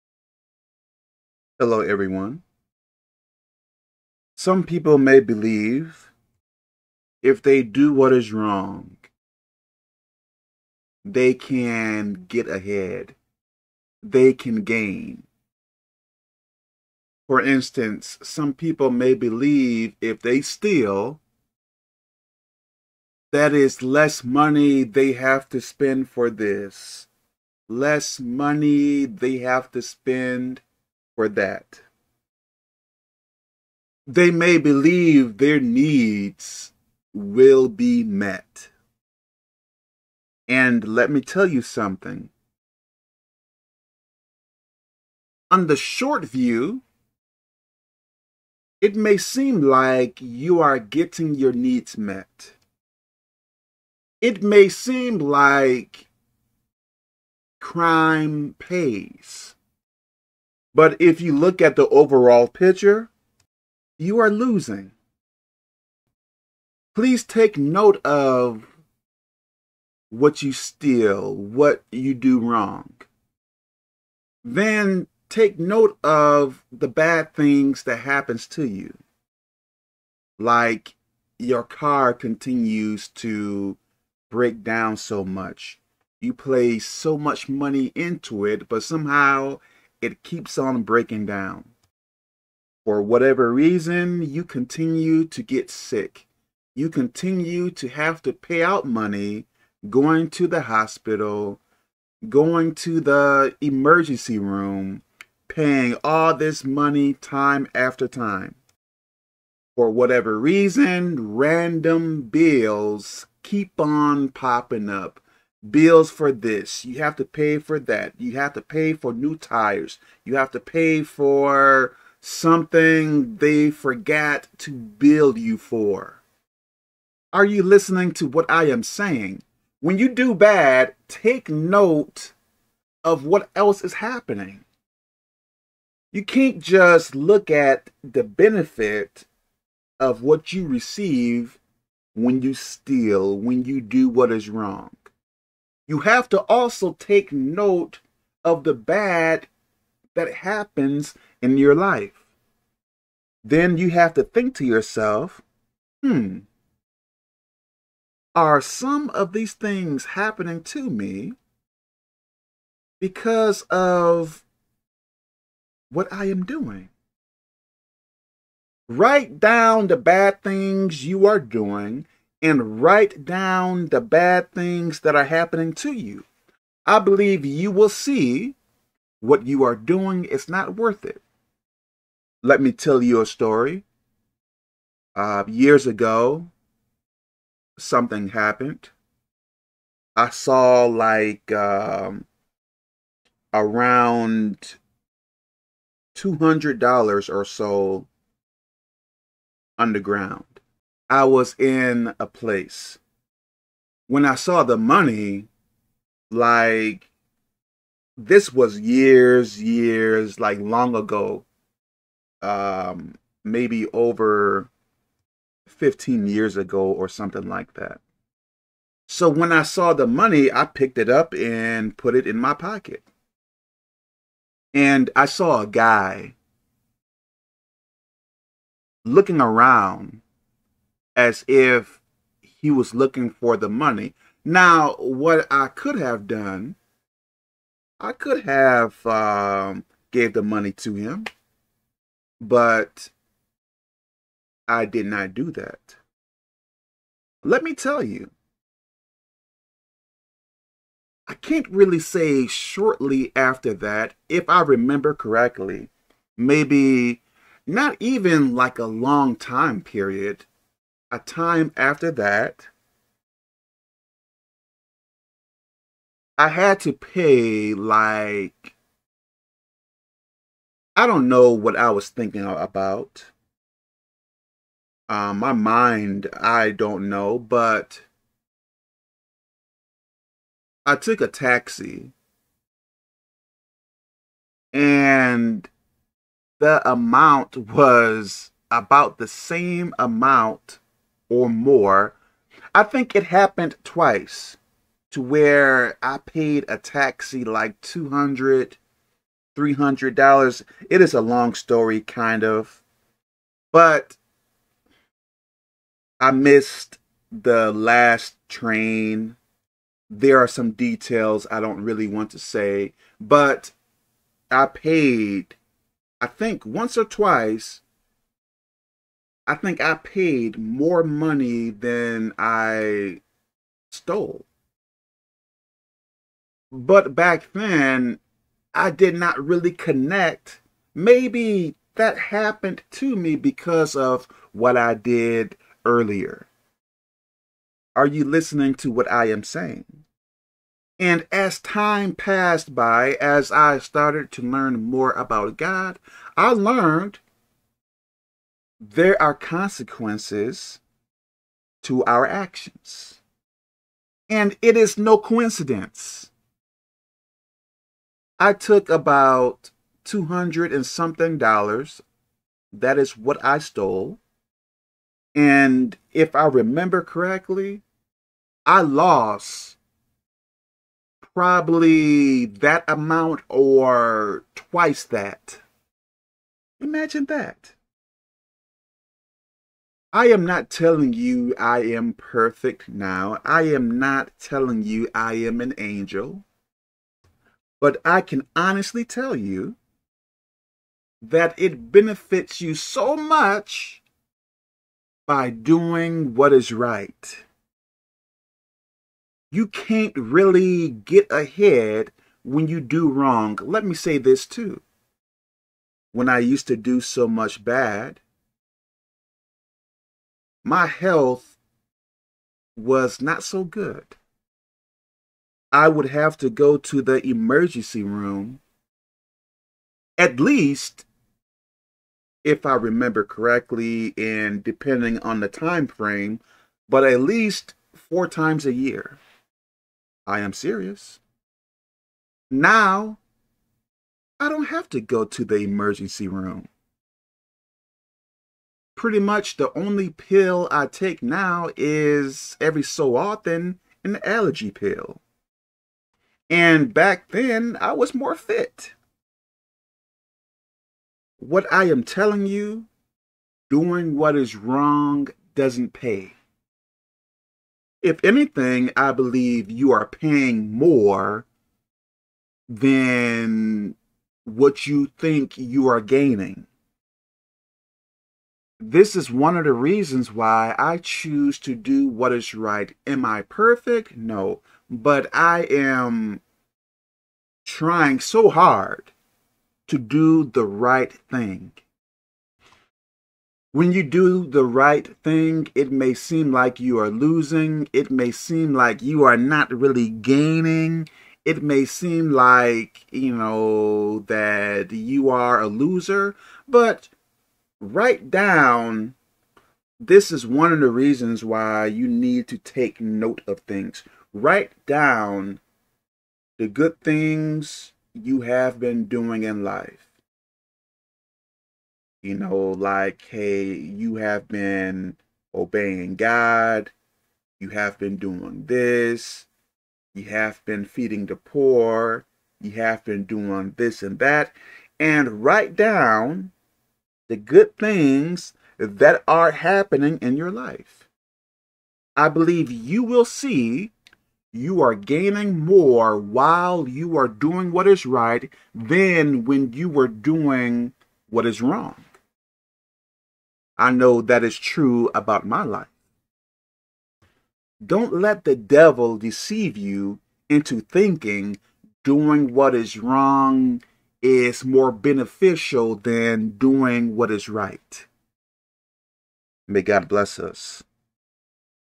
Stealing hurts you more.
Stealing isn't as beneficial as you think it is. You may think it is beneficial because you aren't doing the math. You aren't adding up the bad things that are happening to you. When we do what is wrong, depending on what it is, we will have to pay for it in some type of way. So in a sense, you are not adding up the costs. If you were to add it all up, you would see that stealing is not really benefiting you. It is probably costing you more than what you think you are gaining.

Colossians 3:25 King James Version
25 But he that doeth wrong shall receive for the wrong which he hath done: and there is no respect of persons.

Kev Breeze